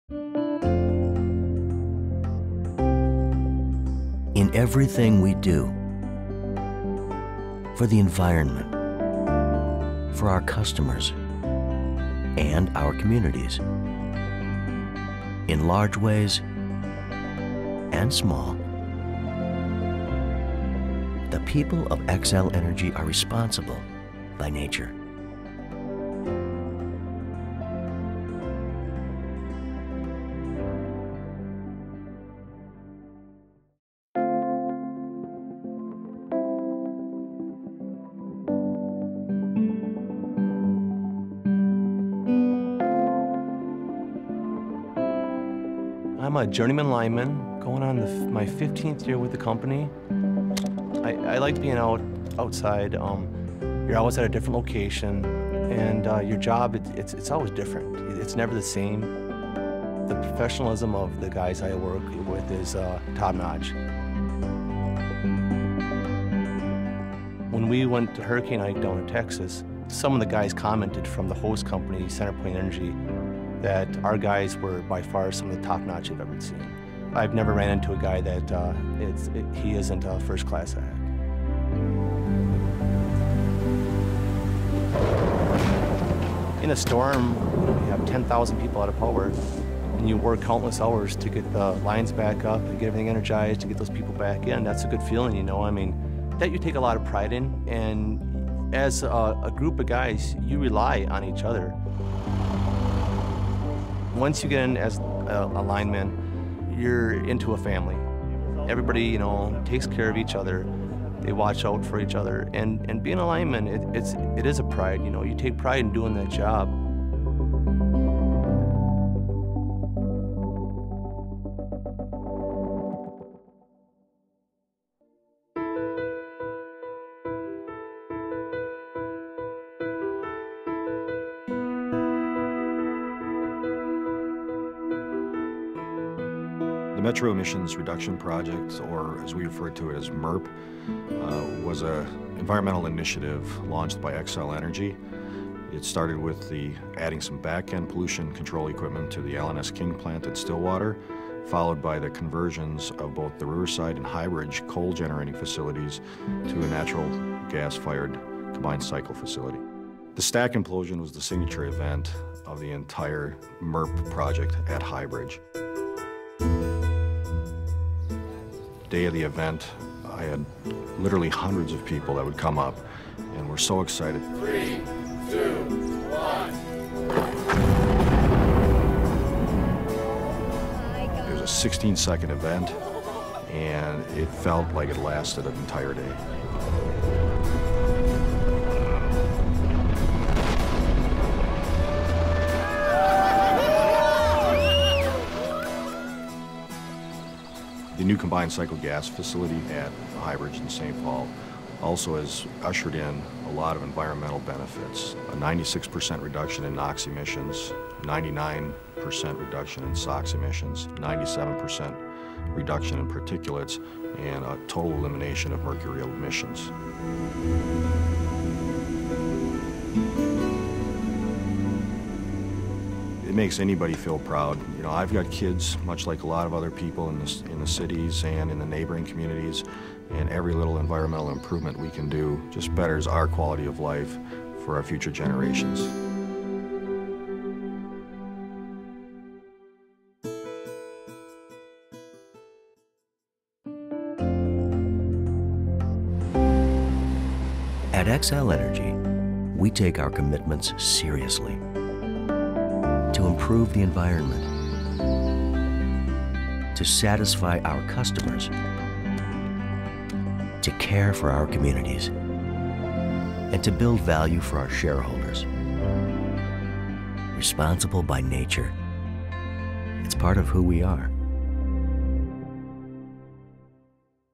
Xcel Energy's Operational Excellence
Employees describe what operational excellence means at Xcel Energy.